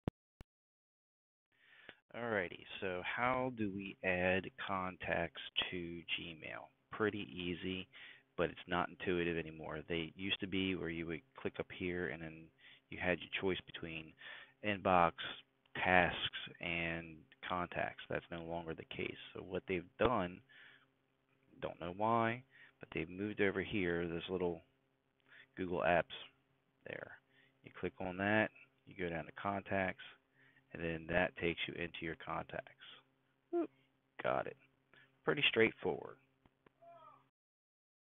How to add contacts to Gmail in 2019
Adding contacts to Gmail has changed with the new update to the interface. If you want to add new contacts and edit them, then this simple but not intuitive method is how you do it.